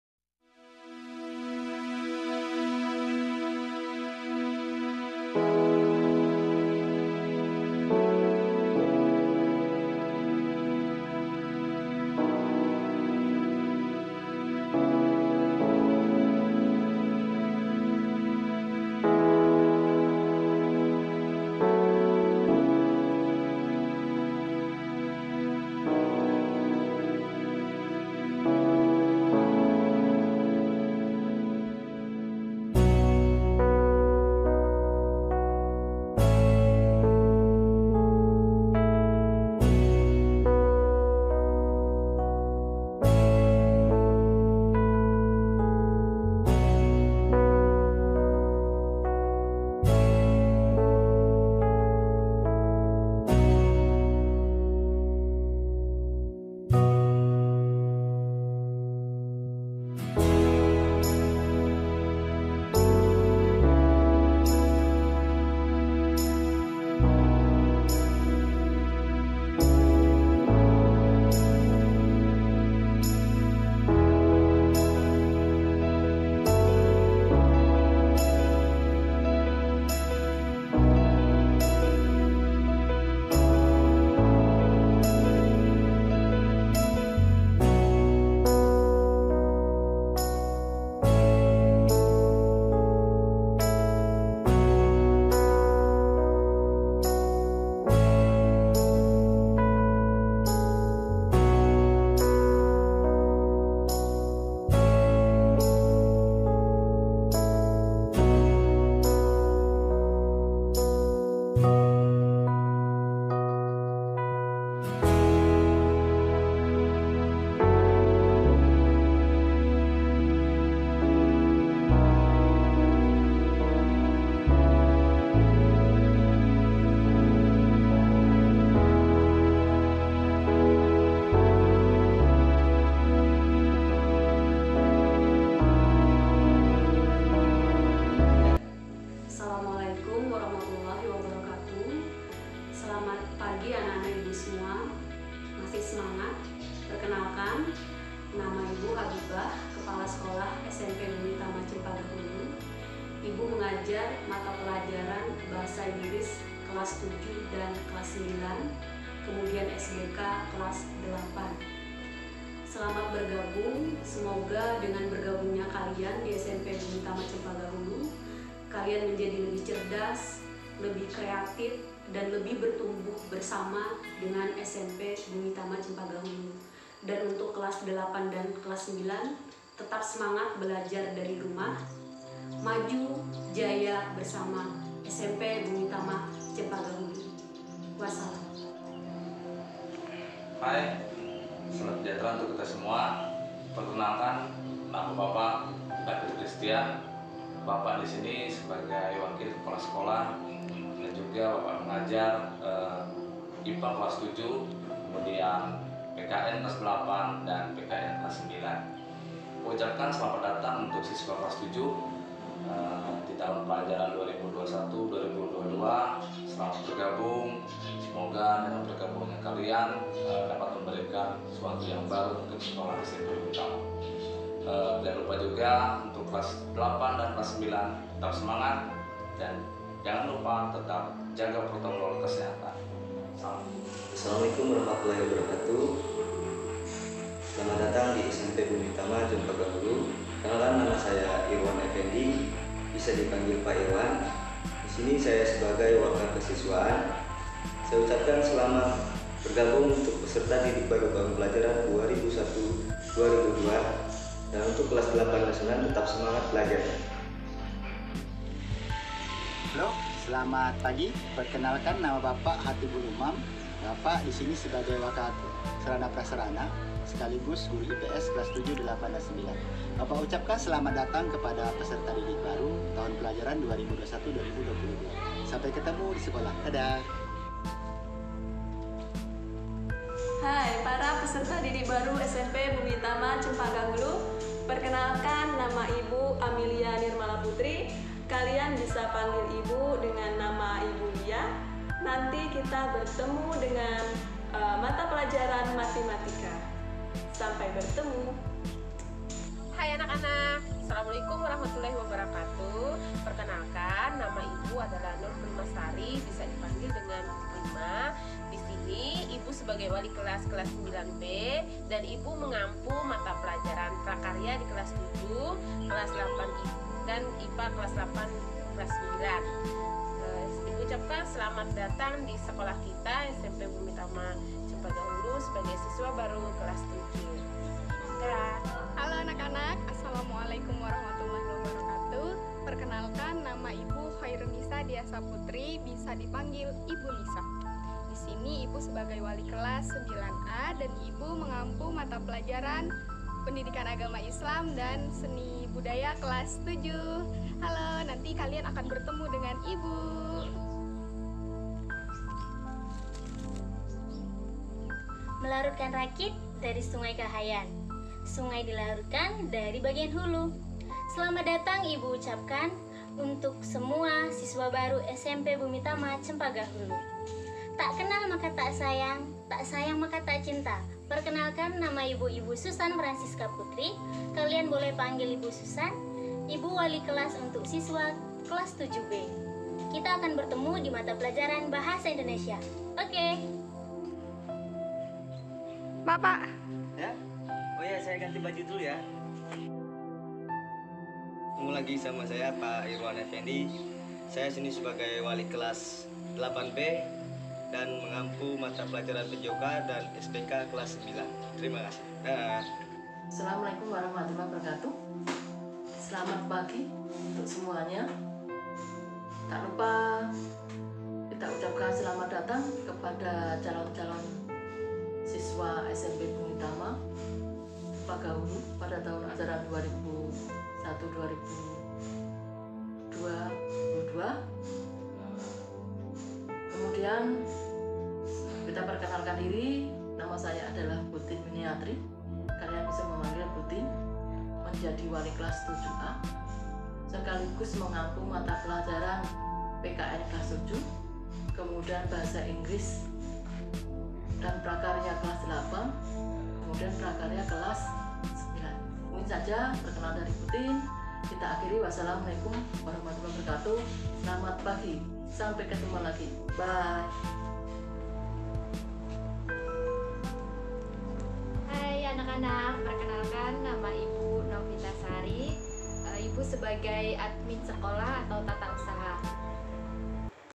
MPLS SMP BUMITAMA CEMPAGA HULU TAHUN PELAJARAN 2021/2022
Asalamualaikum wr.wb
Hai anak-anak semua, kali ini kita upload video MPLS SMP BUMITAMA CEMPAGA HULU TAHUN PELAJARAN 2021/2022.

Semoga video ini dapat menambah wawasan baik untuk siswa/i SMP Bumitama sendiri maupun teman-teman dan semoga video ini dapat membuat kalian mengenal pendidik dan tenaga pendidikan di tahun pelajaran 2021/2033 serta menghapus kerinduan kalian terhadap suasana belajar di sekolah selama masa pendampingan belajar di rumah.

Jangan lupa kunjungi IG kami di

Akhir kata Wasalamualaikum wr.wb.

 "Yayasan bumitama tumbuhkan minat kembangkan bakat berkarakter setulus Hati" .

Model : Semua tenaga Pendidik dan Kependidikan di SMP Bumitama Cempaga Hulu tahun pelajaran 2021/2022

editor : Irwan Efendi